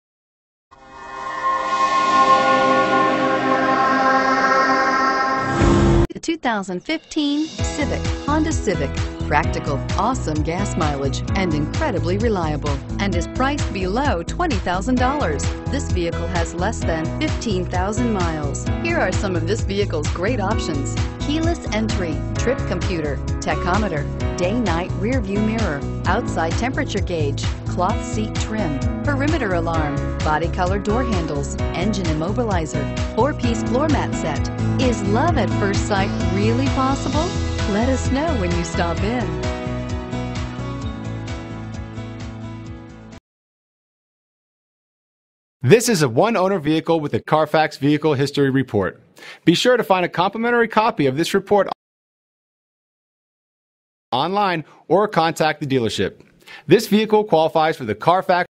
2015 HONDA CIVIC Irving TX FE072109
You'll love this 2015 HONDA CIVIC. This is a  you'll want to take home. With 11786 miles, it features Automatic transmission and an exterior color of . Call and get in touch with David McDavid Honda of Irving directly, and be the first to open the door today!